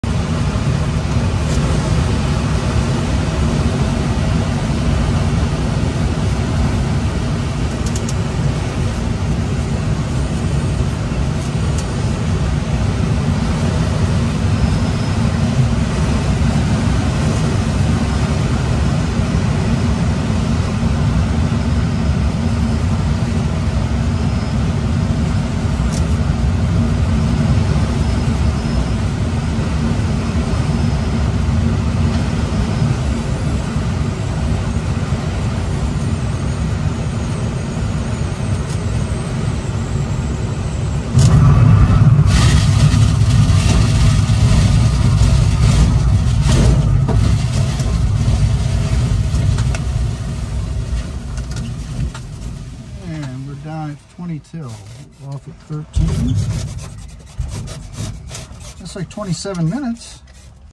Cockpit view of grass strip glider landing
Grass strip landing of a dual passenger, Schweizer SGS 2-33A in Hampshire, Illinois.